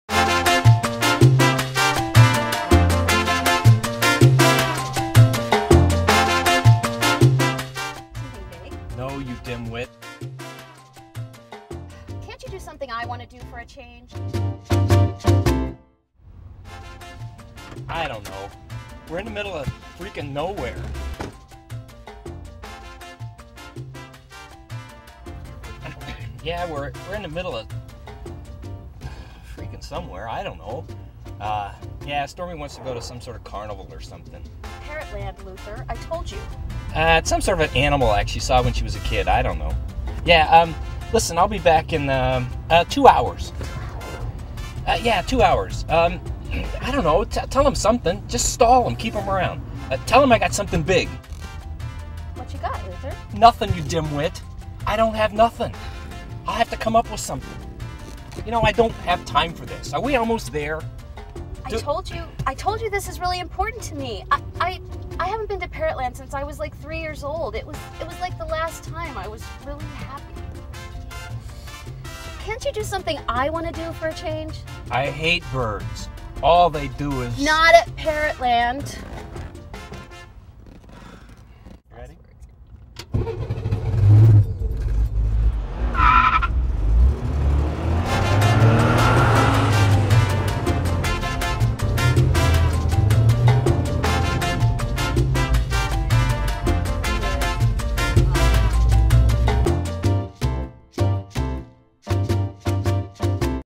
The Adventures of Luther and Stormy
A narrative video edited for Introduction to New Media, a Media Communications class at UAFS.  Edited by Landon Harwell, Leslie Hassel, Megan Jones, and Courtney Moss.